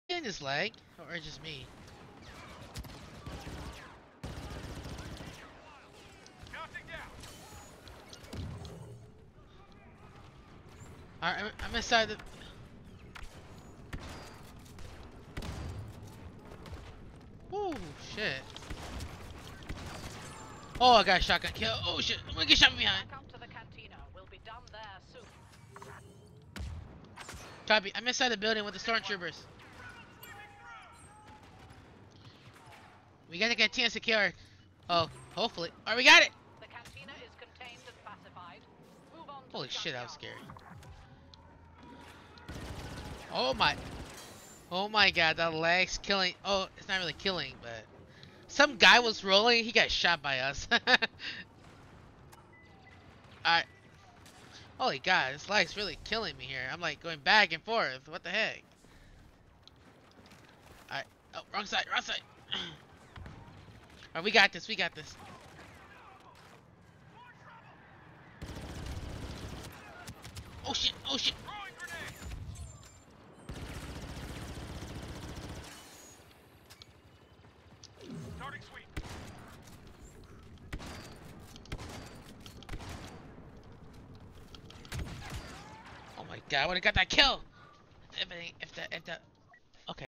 Very Dirty
Imperial forces hold the Cantana and now moving to secure second area. Notice how dirty my stormtrooper armor is, -- Watch live